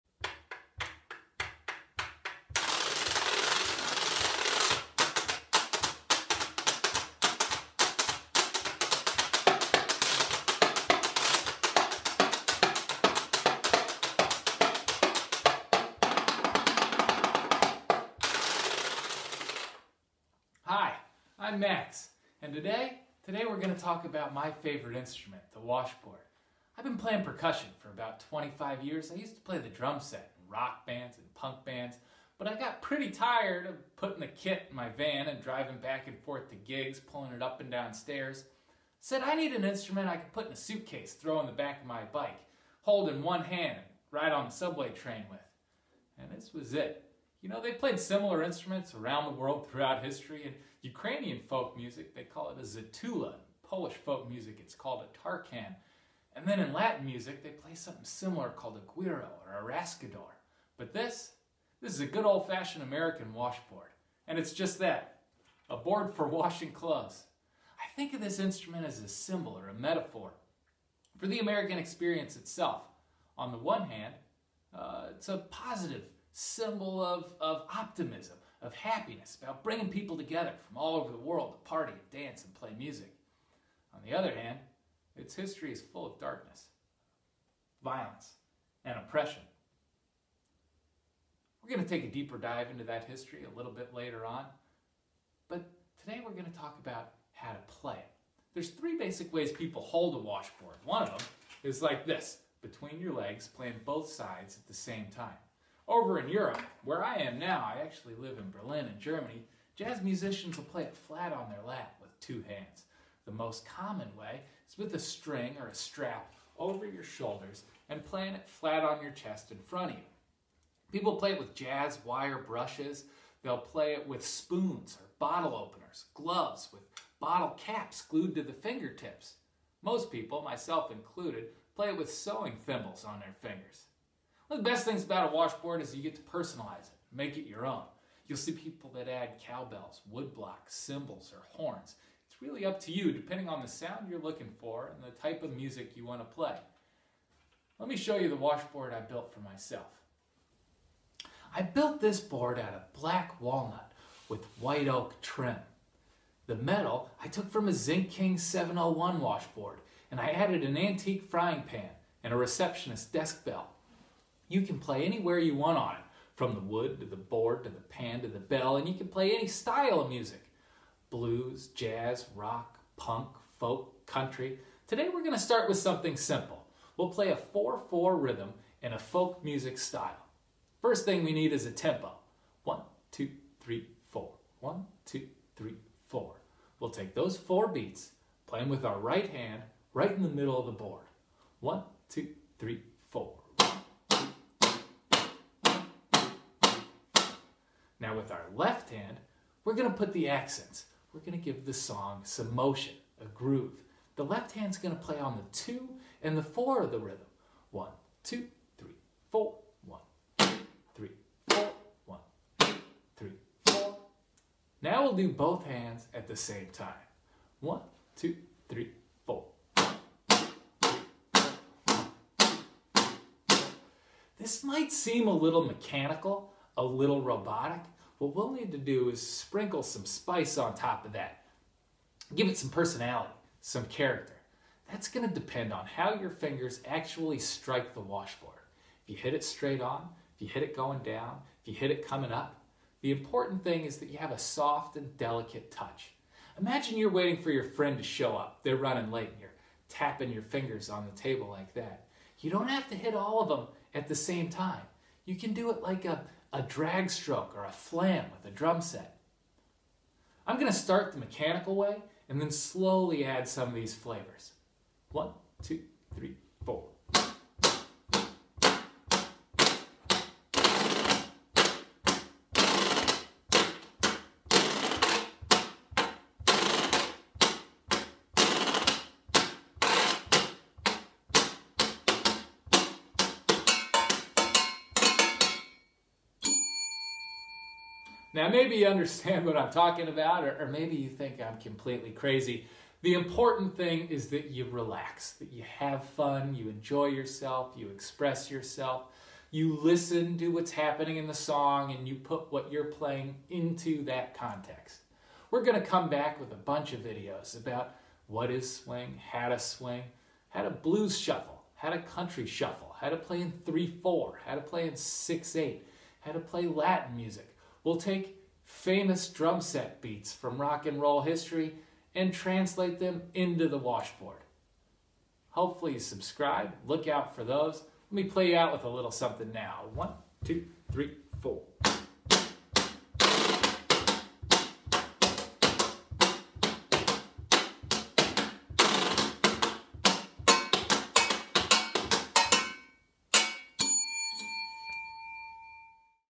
Learn the Washboard 1: Basics
Learn how to play the washboard: I begin with a musical demonstration and then discuss some of the ways people play the washboard around the world. We learn our first basic washboard rhythm, a standard 4/4 folk beat. This rhythm will be the core of everything else we learn moving forward.